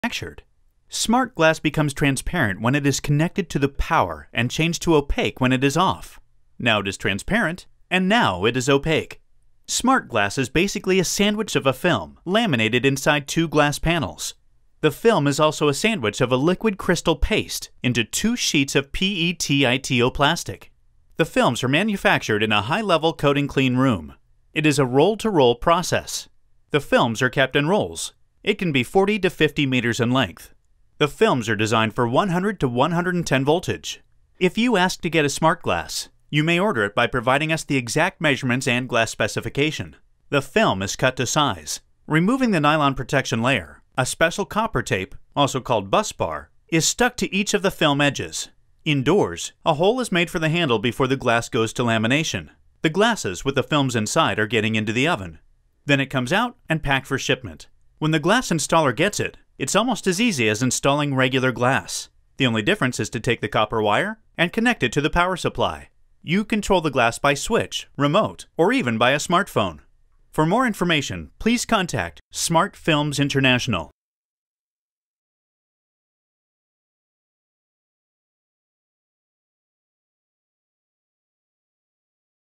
SFI, Smart Glass, what is it really ?
What is it really Privacy Glass? How it is done?